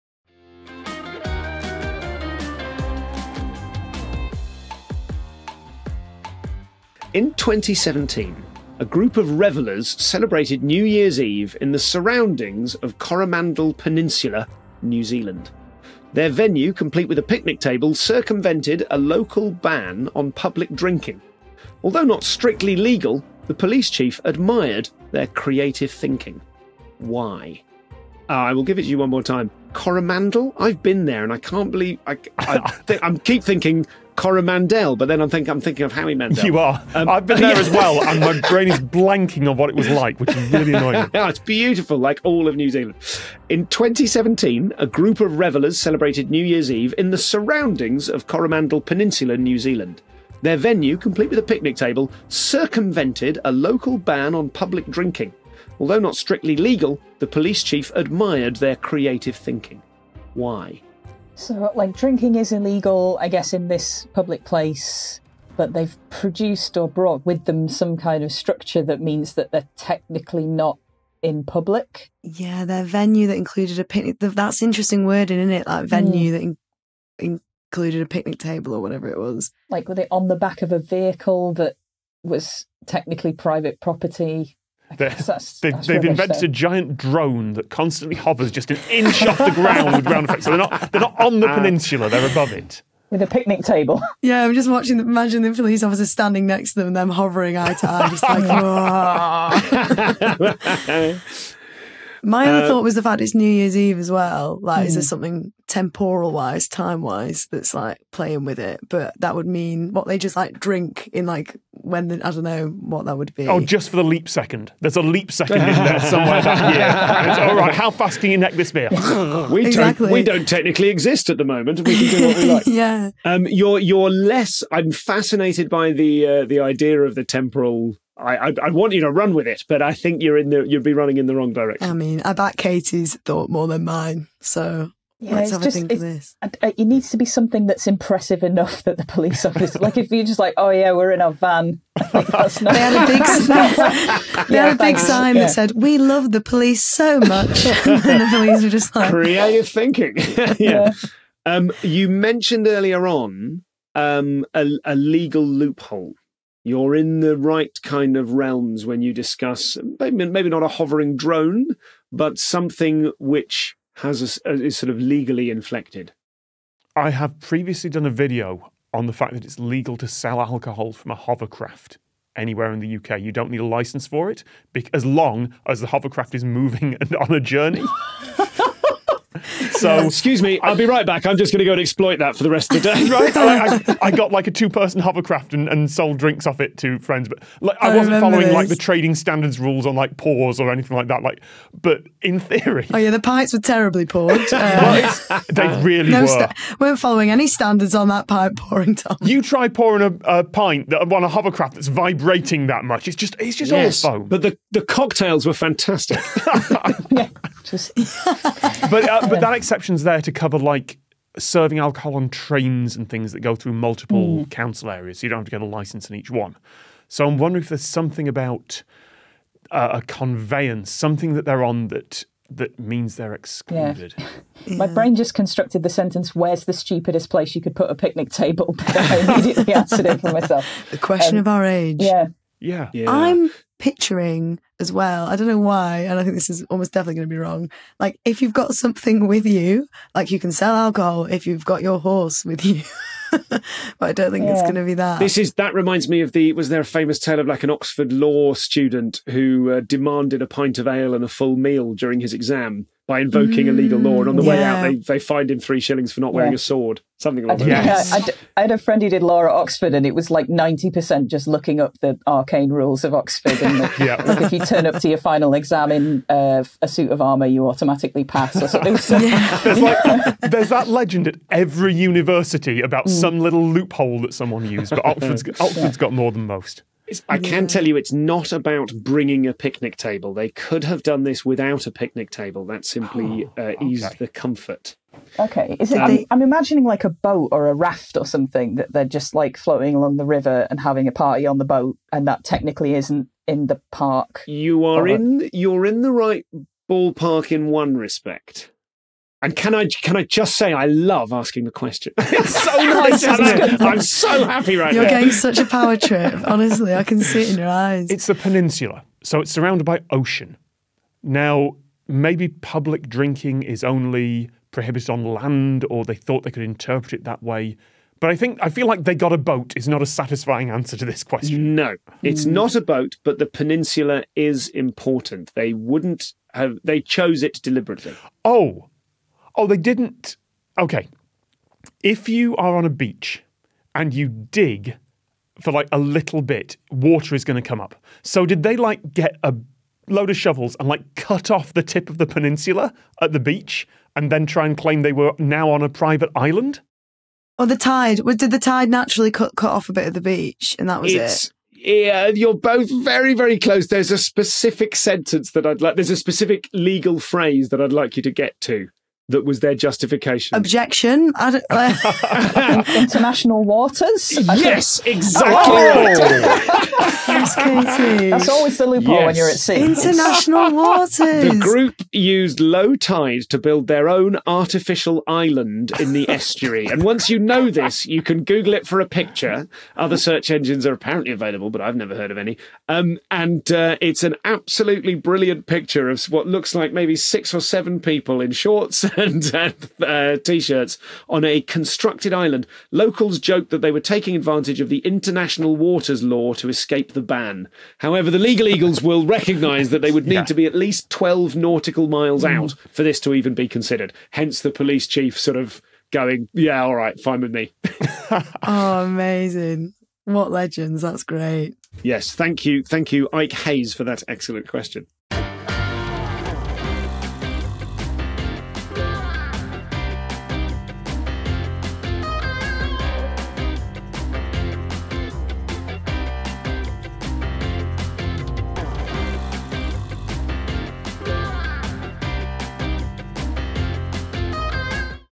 How boozers bypassed this ban
Stuart Goldsmith,  Sophie Ward and Katie Steckles face a question about alcohol ban avoidance.

QUESTION PRODUCER: David Bodycombe.

RECORDED AT: The Podcast Studios, Dublin.
EDITED BY: Julie Hassett.